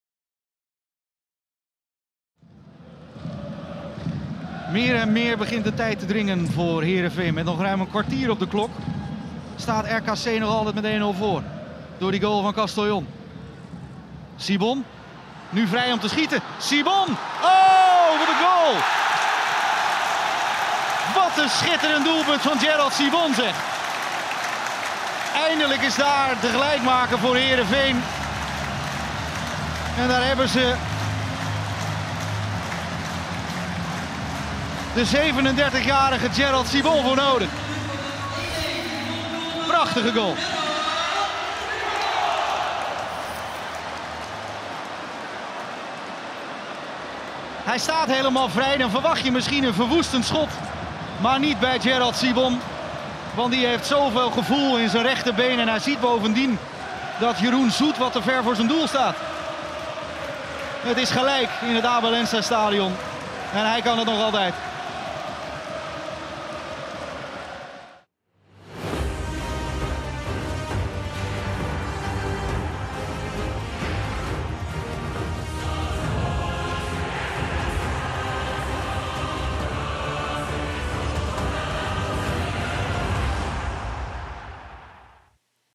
SUBLIEME GOAL Gerald Sibon👌 | sc Heerenveen - RKC Waalwijk (19-11-2011)
Zaterdag 19 november 2011, doelpunt Gerald Sibon tijdens sc Heerenveen - RKC Waalwijk (1-1).